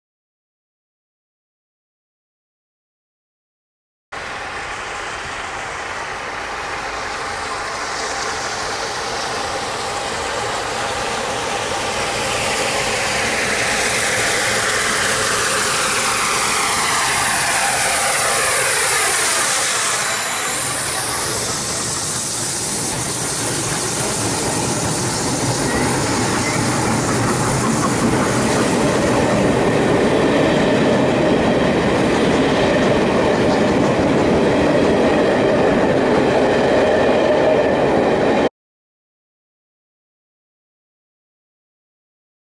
Vintage Trains The Shakespeare Express Week 1 21/07/2019
7029 on Vintage Trains The Shakespeare Express, 1T51 1241 Stratford-upon-Avon to Birmingham Snow Hill, seen passing Olton.